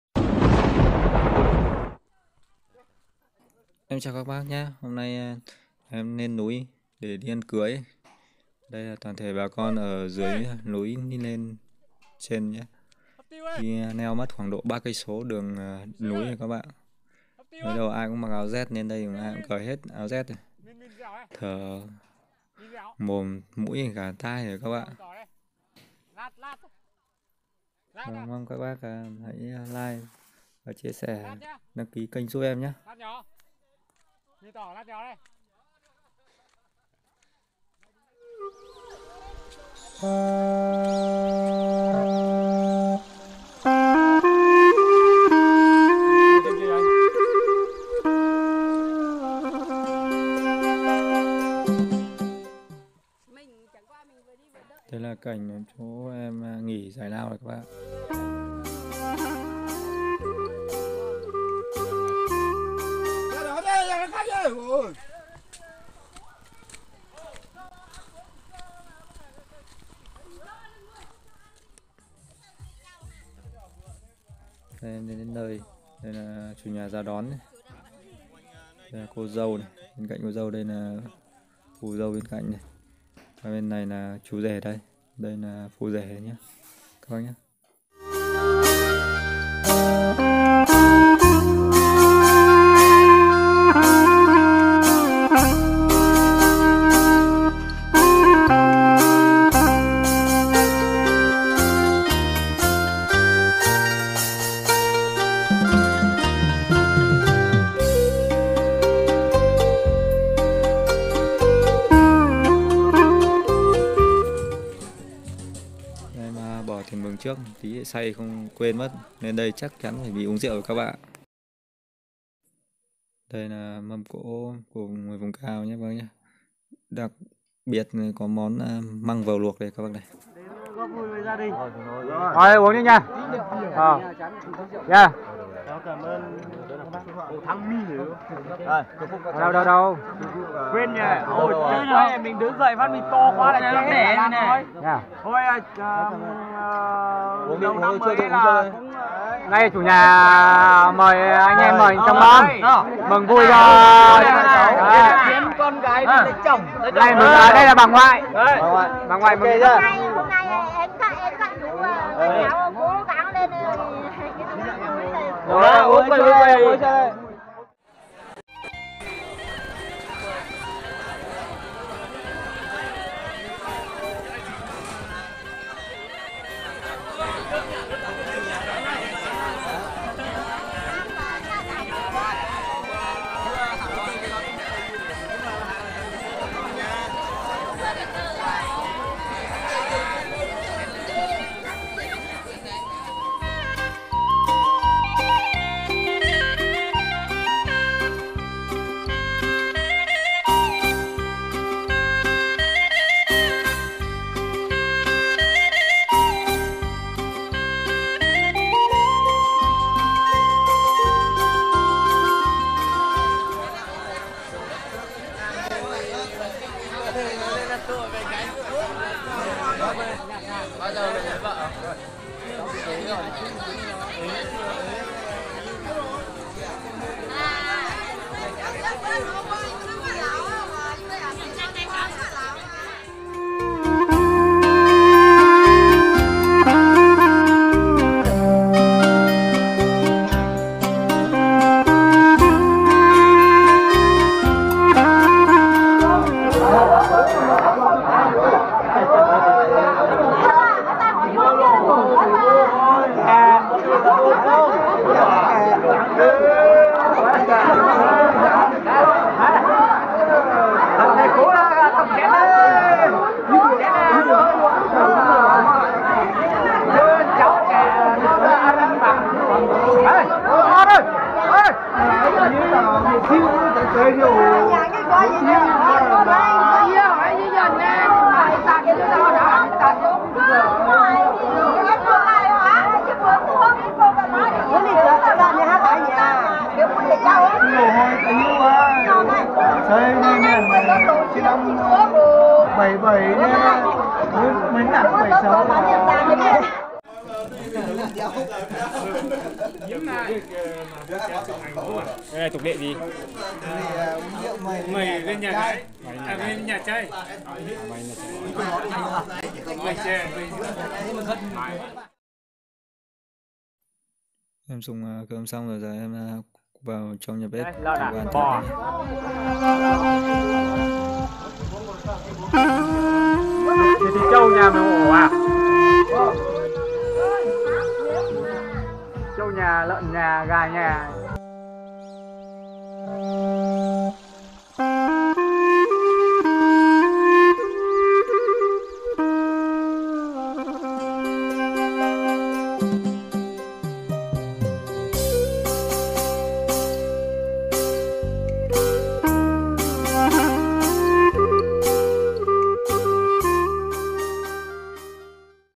LÊN NÚI ĂN CƯỚI NGƯỜI DÂN TỘC DAO
Leo bộ 3km lên đỉnh núi của bà con dân tộc người dao đỏ rất hay.
Nợ TV đam mê xê dịch trên những cung đường, và tìm hiểu văn hóa rất mong anh em,bạn bè, quý cô bác ủng hộ kênh, xin chân thành cảm ơn